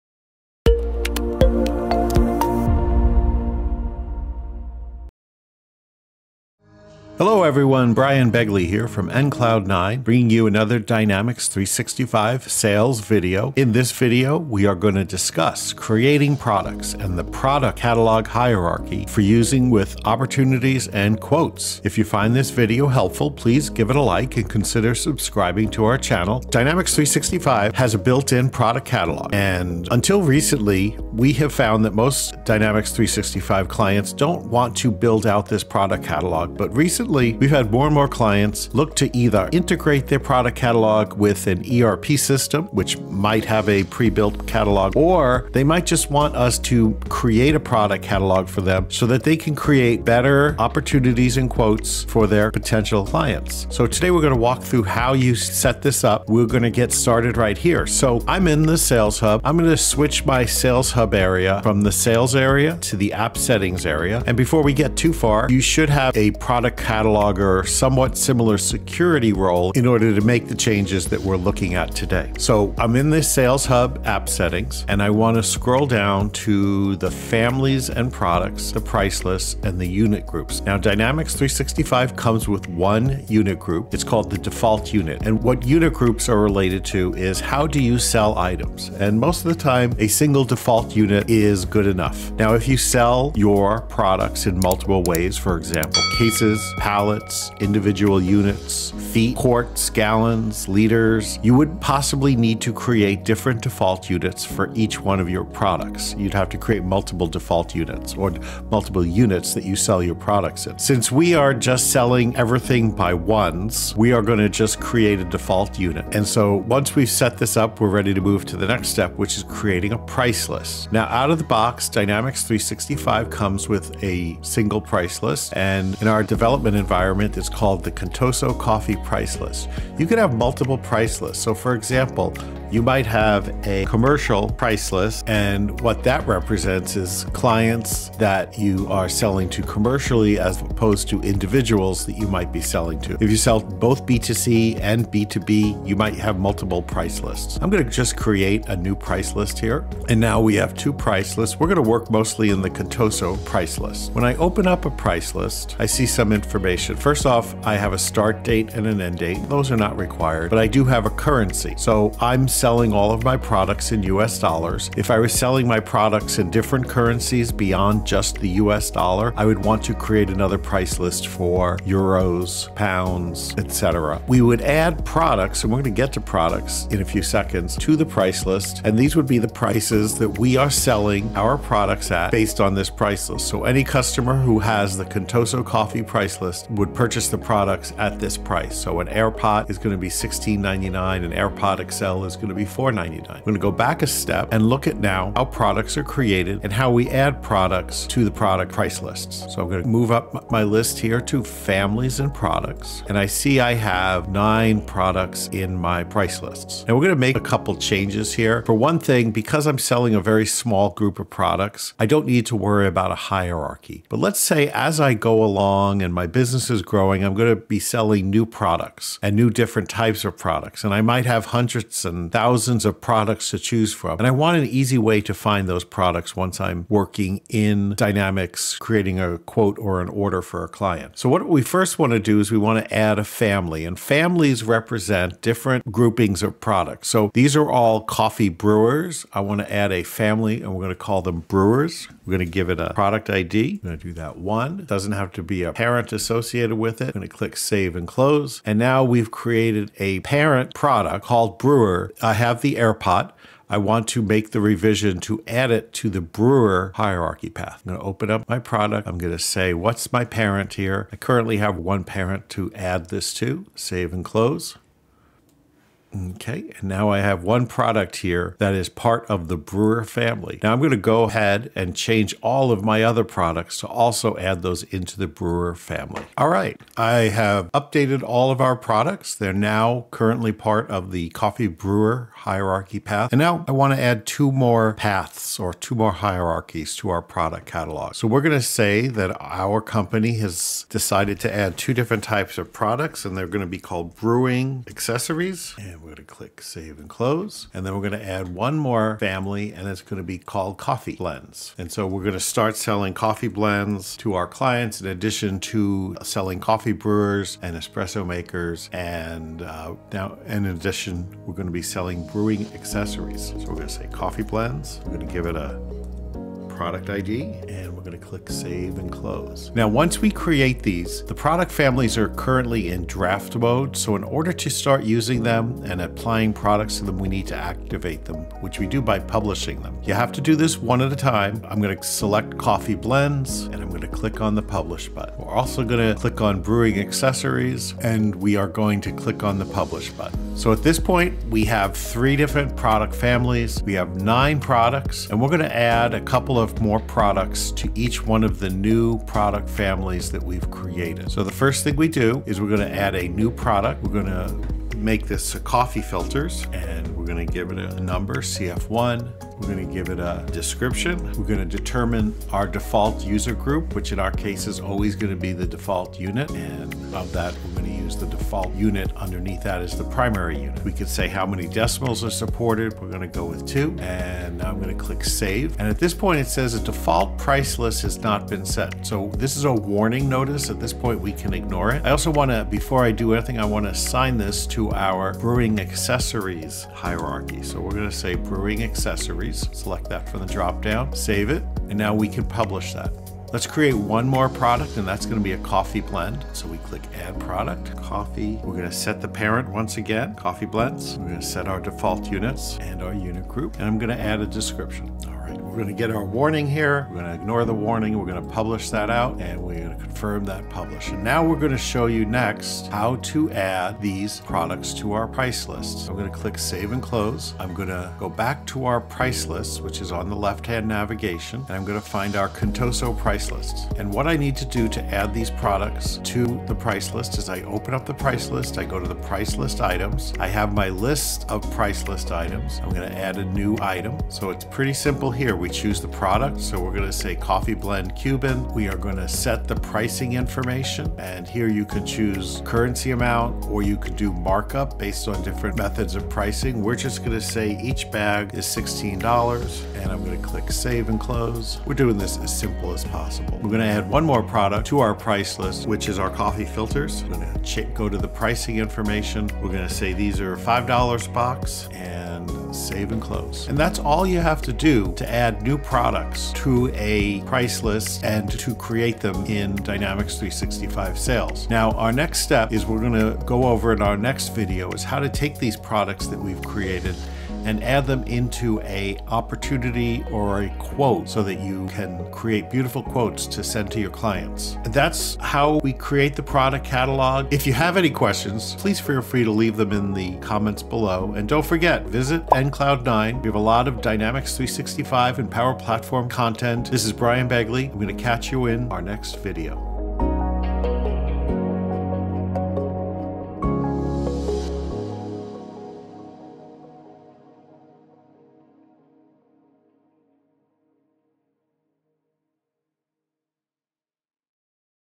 How to Add Your Product Catalog to Dynamics 365 Sales | enCloud9
Want to build a complete product catalog in Dynamics 365 Sales? Schedule a quick call with enCloud9 today:

Building a complete product catalog in Dynamics 365 Sales helps your team create accurate quotes, track pricing, and streamline opportunity management. In this step-by-step tutorial, we’ll show you how to set up your product catalog, add product families, assign pricing, and structure everything for easy use in opportunities and quotes.

✔️ Learn how to configure unit groups and price lists
✔️ Create and organize products into product families
✔️ Assign default units and pricing for each product
✔️ Publish product hierarchies for use in quotes and orders
✔️ Add products to price lists and prepare them for sales use

Whether you’re integrating with an ERP system or building from scratch, this video will help you fully set up your Dynamics 365 product catalog the right way.

Timestamps:
0:00 – Intro
0:26 – Why use the Product Catalog in Dynamics 365
1:04 – Security roles needed
1:20 – Understanding unit groups
2:00 – Creating and managing price lists
3:19 – Adding products to the catalog
3:57 – Creating product families (e.g., Brewers)
5:01 – Creating additional families (Accessories, Coffee Blends)
5:29 – Publishing product families
5:51 – Creating new products and assigning them to families
6:55 – Adding products to price lists
7:51 – What’s next: adding products to opportunities and quotes